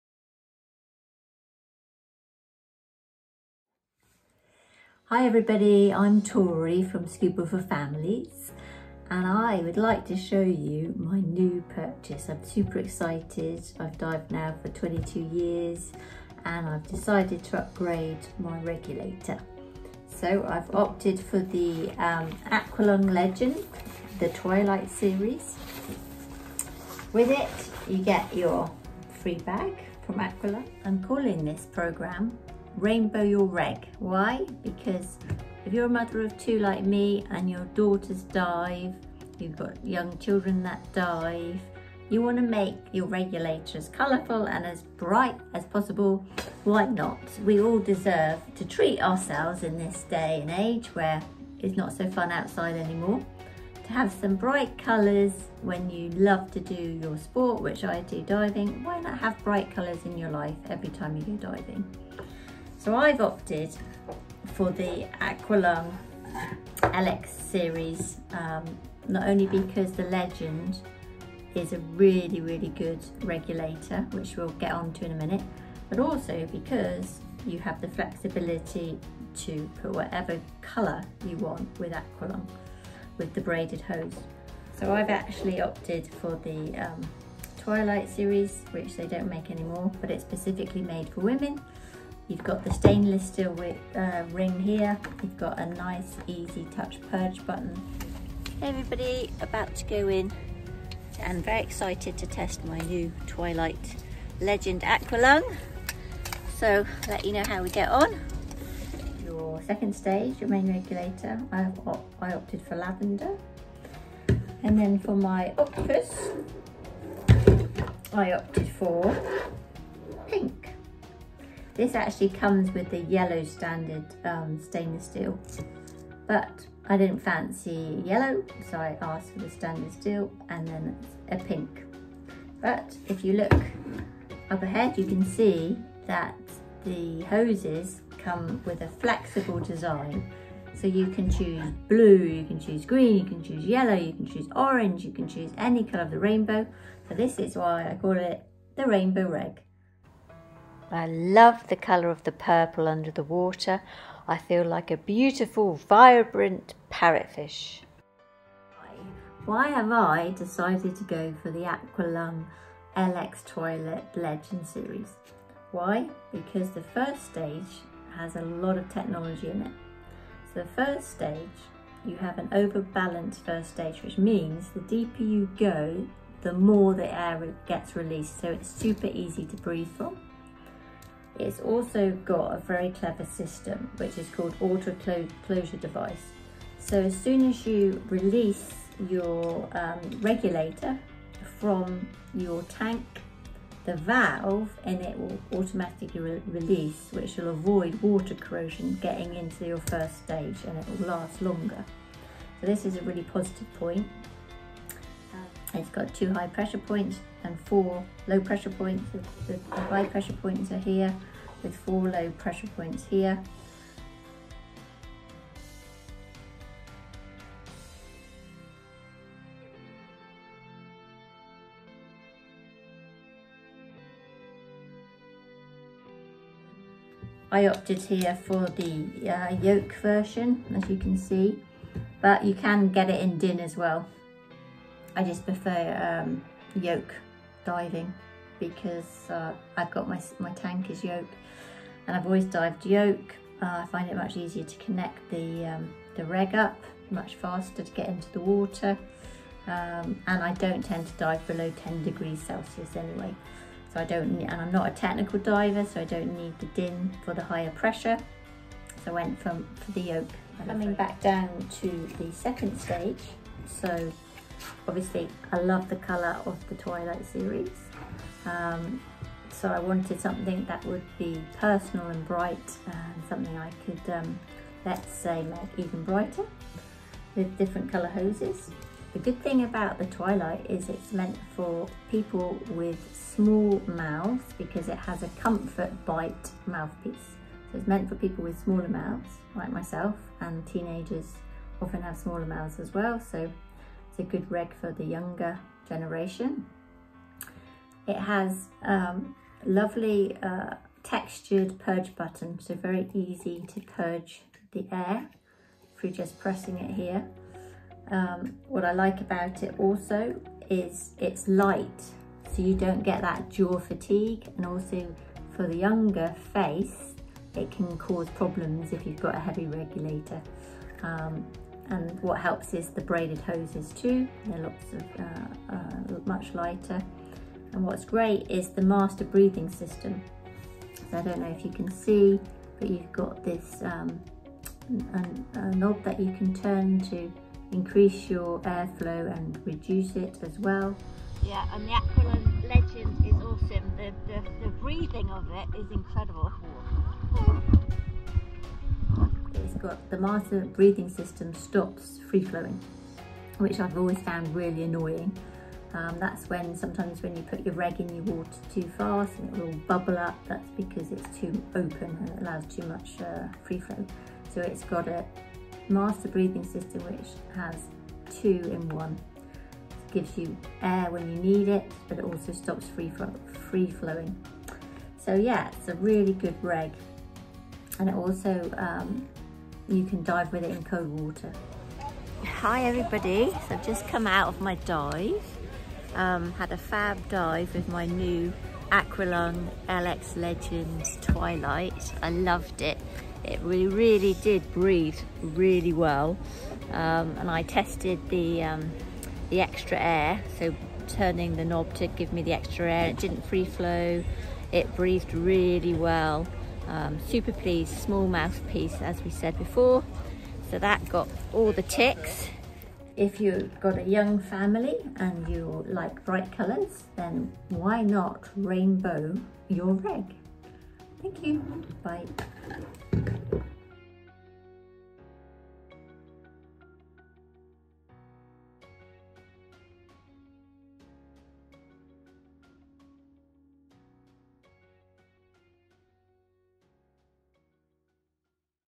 Best Ladies Scuba Regulator - Aqualung Legend LX ACD Supreme -Twilight Edition - Rainbow your Reg
A full review of the Aqualung Legend LX ACD SUPREME regulator - TWILIGHT Edition
Why we think its the best ladies or teenager regulator out there. Also a guideline on how to "Rainbow Your Reg" to bring in the much needed colour into our lives.

The Twilight Legend LX includes a smaller Comfo-Bite™ mouthpiece to reduce jaw fatigue designed specifically with women or young people in mind. The Legend LX Twilight also features the revolutionary Master Breathing System (MBS) for simple breathing comfort control, and its own regulator bag for safe and stylish storage.